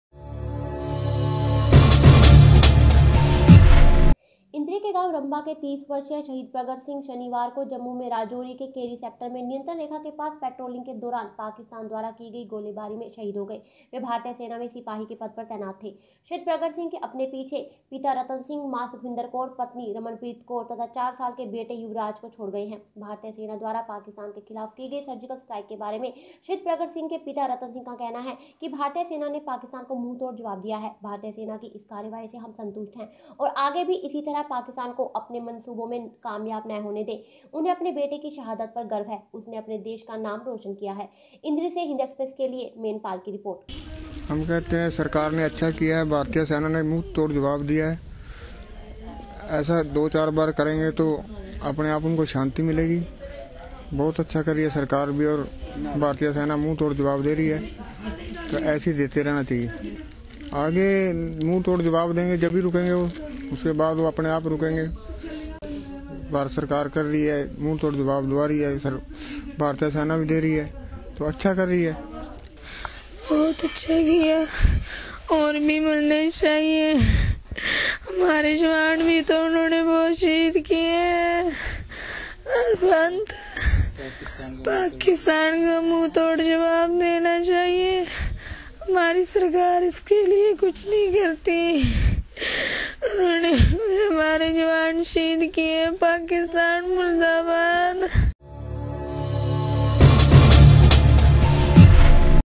भारतीय सेना के सर्जिकल स्ट्राइक से संतुष्ट शहीद का परिवार
इंद्री के गांव रम्बा के 30 वर्षीय शहीद प्रगट सिंह शनिवार को जम्मू में राजौरी के केरी सेक्टर में नियंत्रण रेखा के पास पेट्रोलिंग के दौरान पाकिस्तान द्वारा की गई गोली बारी में शहीद हो गए। वे भारतीय सेना में सिपाही के पद पर तैनात थे। शहीद प्रगट सिंह अपने पीछे पिता रतन सिंह, माँ सुखविंद्र कौर ,पत्नी रमनप्रीत कौर तथा चार साल के बेटे युवराज को छोड गये है। भारतीय सेना द्वारा पाकिस्तान के खिलाफ की गई सर्जिकल स्ट्राइक के बारे में शहीद परगट सिंह के पिता रतन सिंह का कहना है कि भारतीय सेना ने पाकिस्तान को मुँह तोड़ जवाब दिया है। भरतीय सेना की इस कार्यवाई से हम सन्तुष्ट है और आगे भी इसी तरह पाकिस्तान को अपने मनसूबों में कामयाब न होने दे। उन्हें अपने बेटे की शहादत पर गर्व है। उसने अपने देश का नाम रोशन किया है। वहीँ इस बारे में शहीद की पत्नी ने कहा कि भारतीय सेना व् देश के प्रधानमंत्री को पाकिस्तान को मुँह तोड़ जवाब देना चाहिए,ताकि आगे भी पाकिस्तान ऐसी हरकतें न करे। उन्होंने कहा कि हमे अपने जवानों पर नाज है।